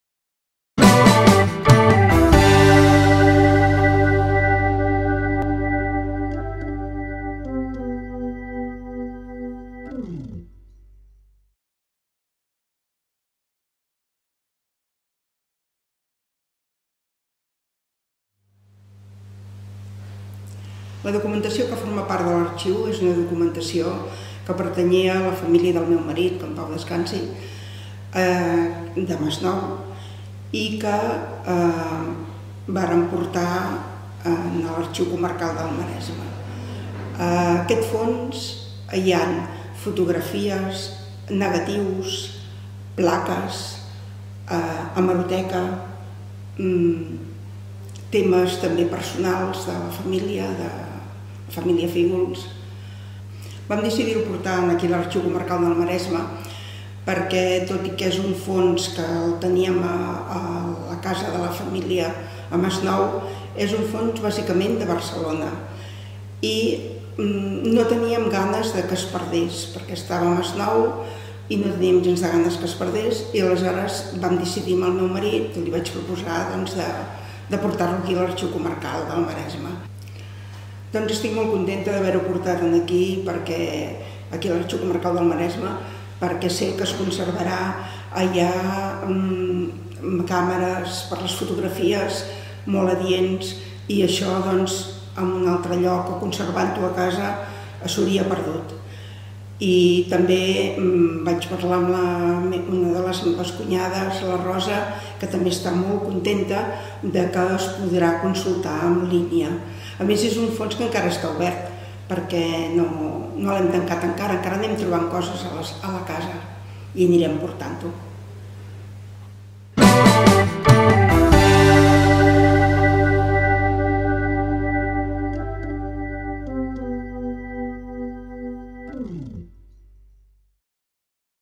Persones - Arxius - Vides: El fons Fíguls-Sallés del Masnou
Setmana Internacional dels Arxius 2021.

El fons Fíguls-Sallés del Masnou (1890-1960) es nodreix d'un gran volum de fotografies (especialment, negatius estereoscòpics i acetats) i de postals. També conté hemeroteca, diplomes, dibuixos, impresos i documentació familiar.
Majoritàriament, bona part d'aquest fons prové de l'arxiu personal de Josep Fíguls Sallés, associat de l'Agrupació Mútua del Comerç i la Indústria de Barcelona.